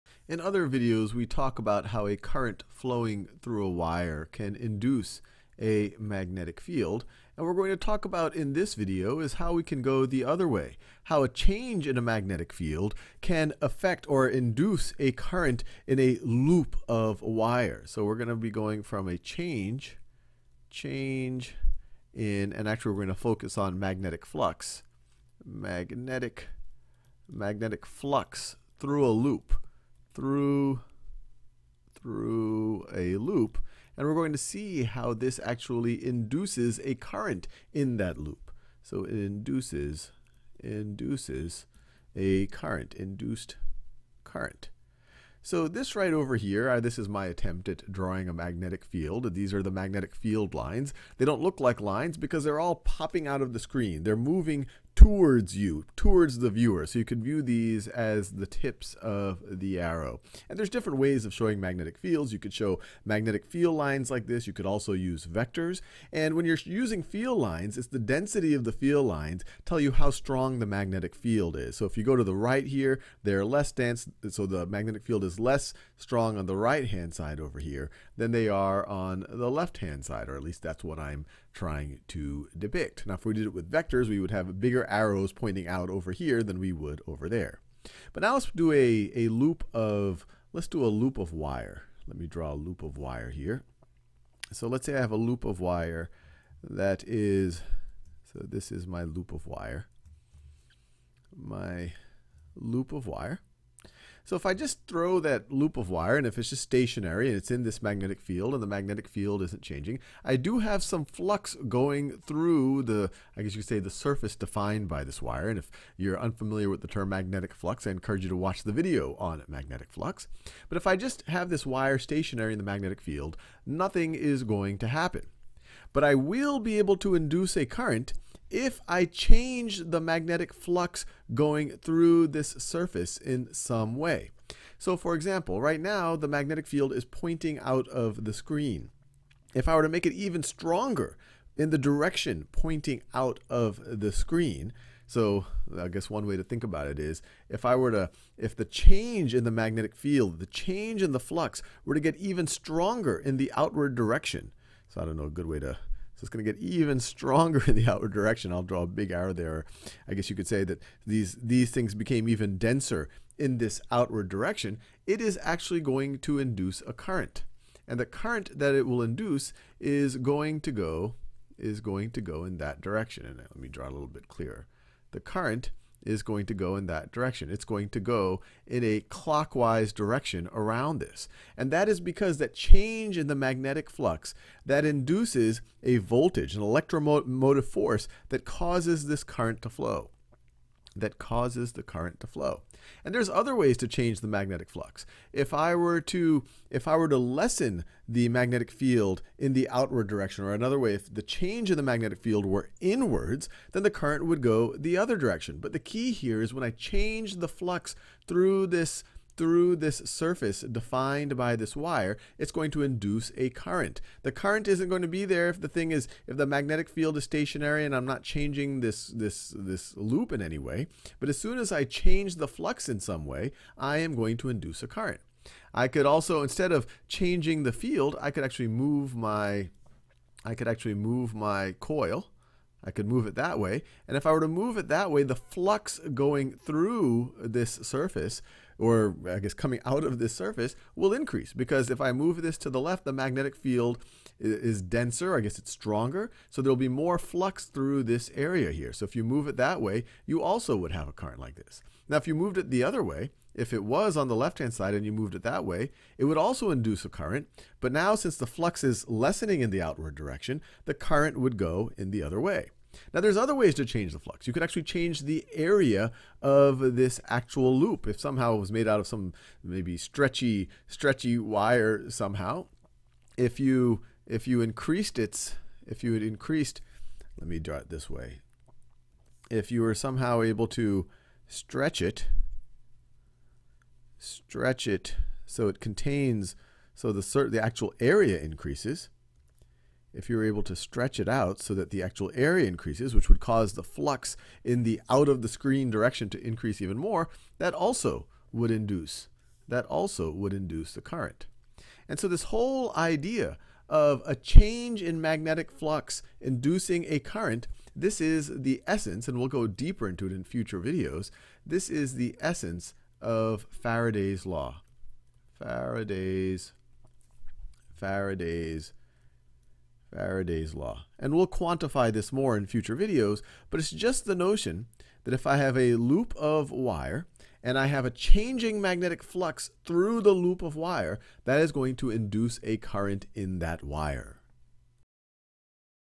Faraday's Law Introduction | Physics | Khan Academy
Courses on Khan Academy are always 100% free. Start practicing—and saving your progress—now

How a current can be induced in a loop of wire by a change in magnetic flux.